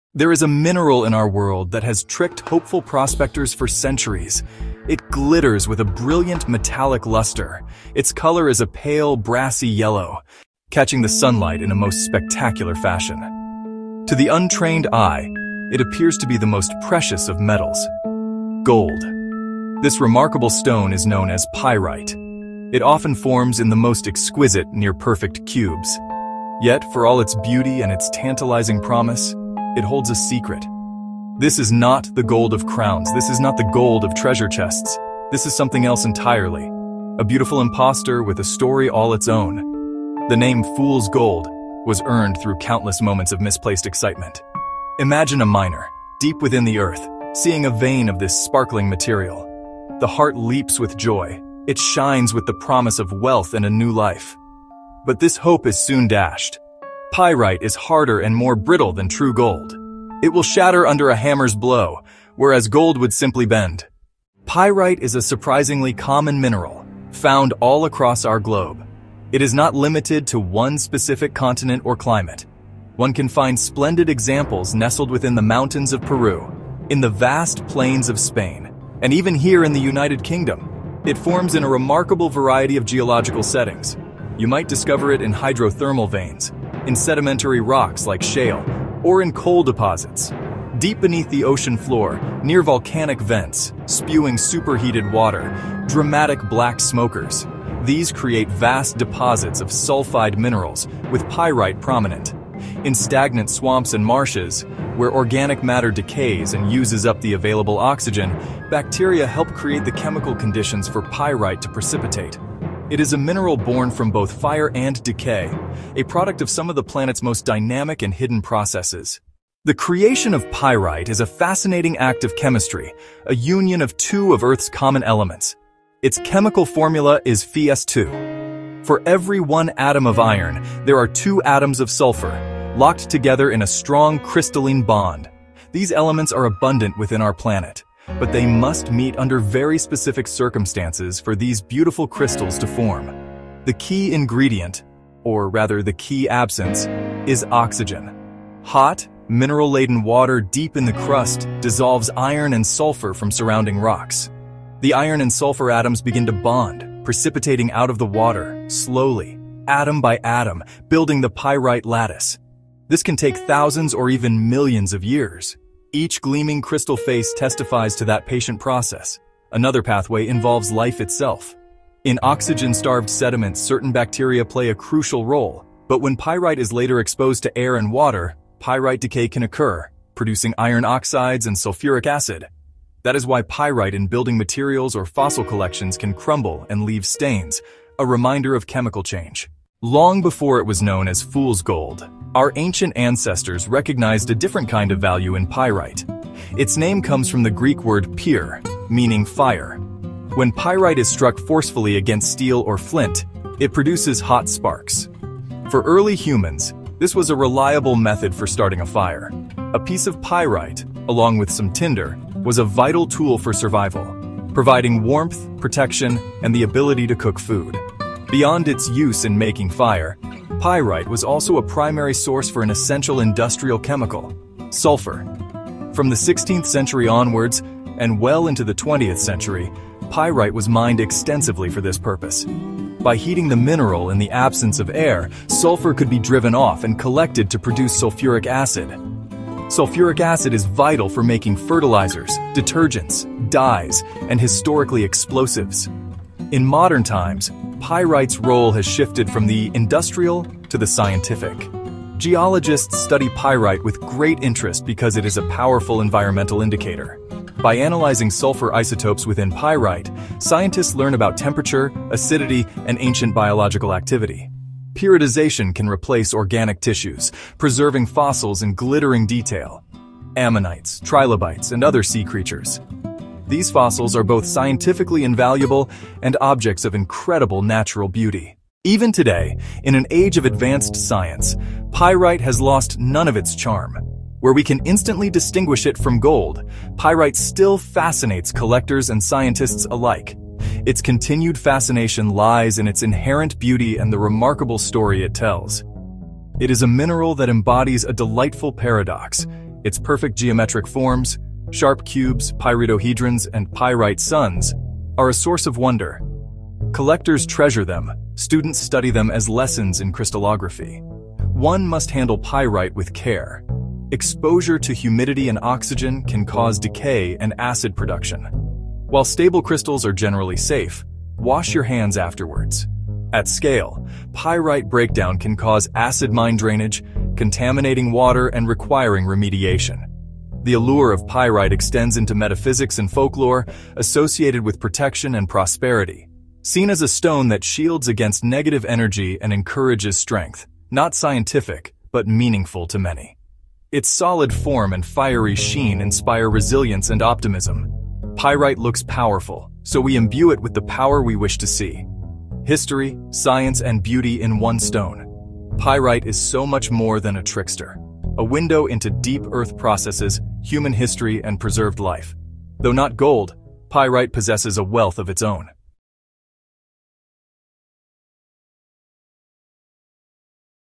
Pyrite Like Never Before: Stunning Visuals & Science
Dive into Pyrite Like Never Before: stunning close-ups, slow-motion spins, and crisp explanations of how this "fool’s gold" forms and sparkles. This 5-minute visual essay blends premium cinematography with clear science — crystal structure, formation environments, and collecting tips — all presented with elegant, museum-quality visuals. All imagery features Pyrite (white crystal substituted where needed) to keep the focus on texture, luster, and microstructure. Perfect for mineral lovers, geology students, and anyone curious about crystals.

OUTLINE:

00:00:00 An Introduction to Fool's Gold
00:00:10 Where Pyrite Resides
00:01:04 The Genesis of Pyrite
00:01:59 The Genesis of Pyrite
00:03:09 Pyrite's Human Story
00:04:38 Why Pyrite Still Captivates